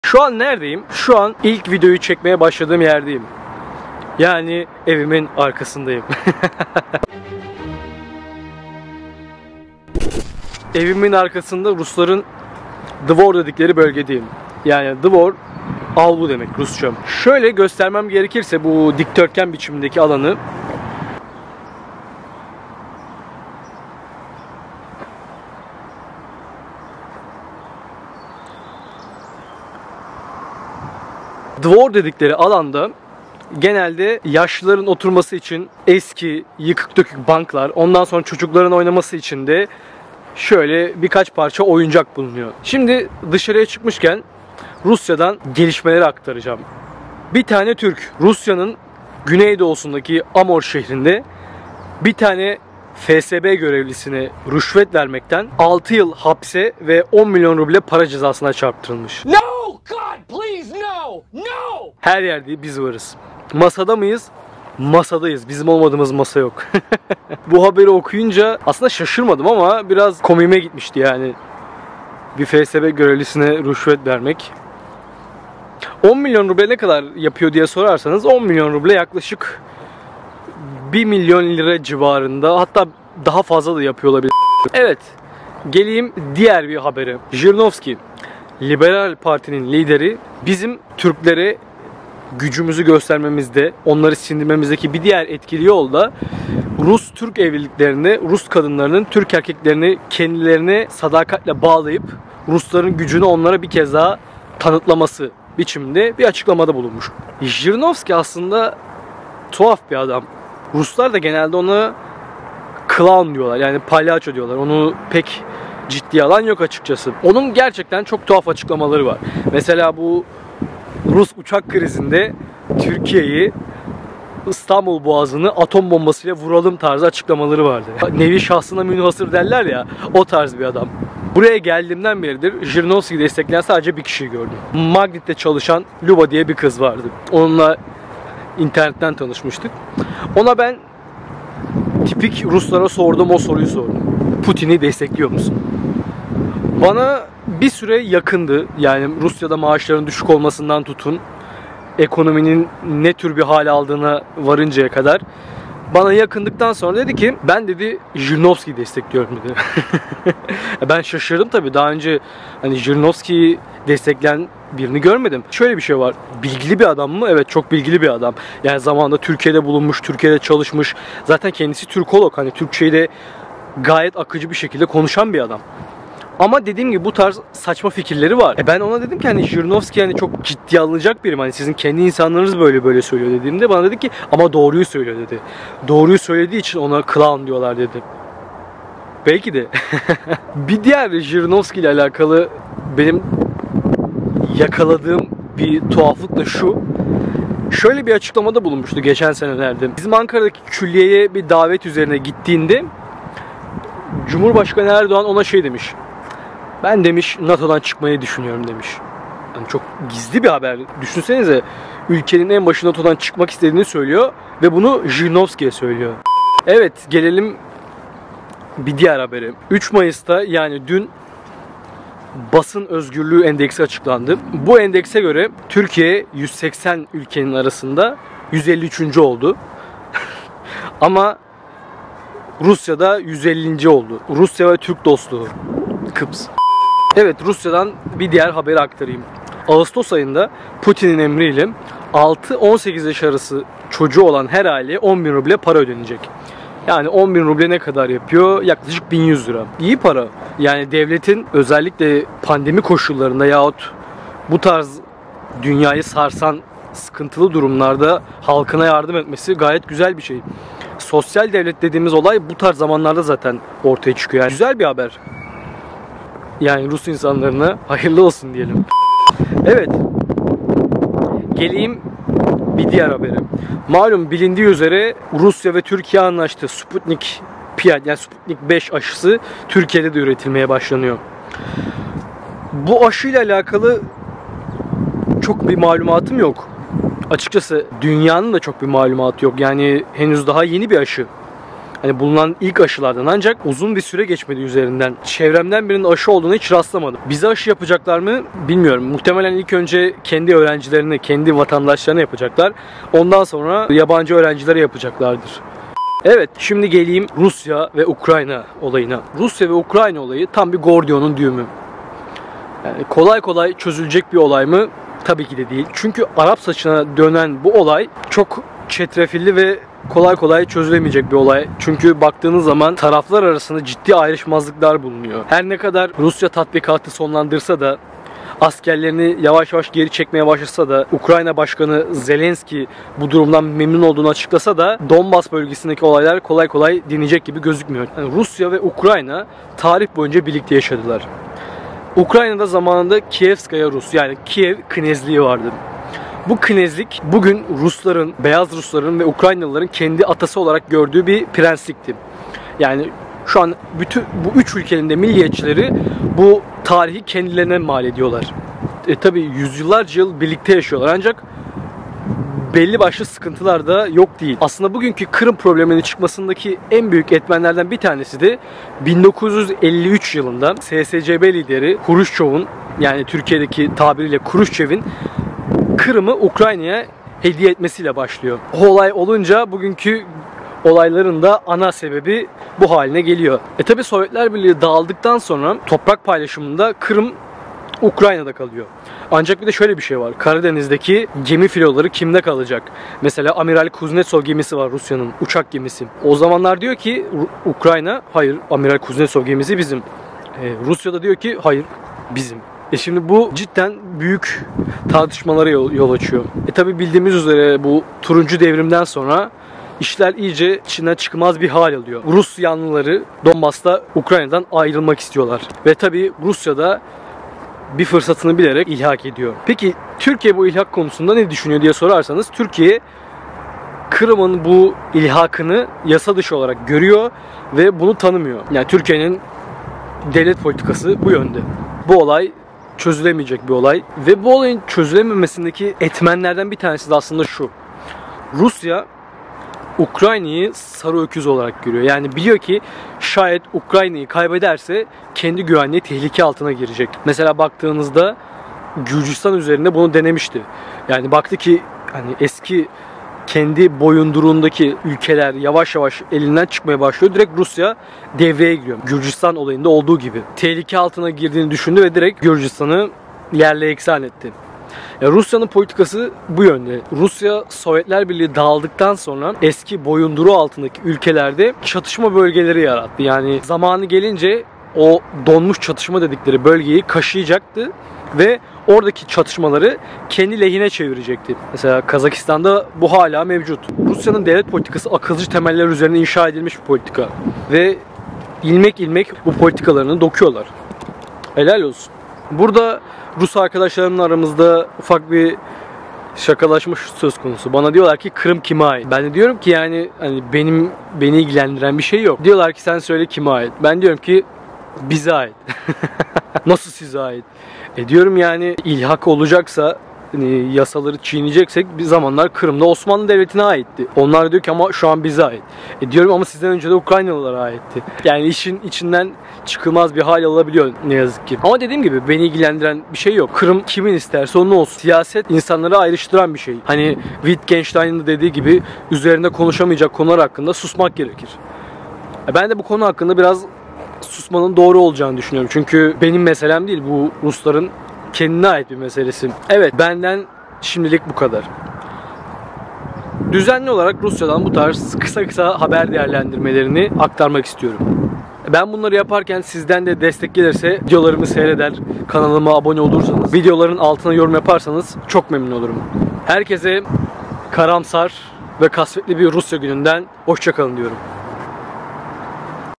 Rusya Gündemi (Sputnik V Aşısı, Rusya ve Ukrayna Krizi)
Herkese merhaba ben Berkay Uğur. Bu videomda Rusya'da öne çıkan haberleri değerlendirdim.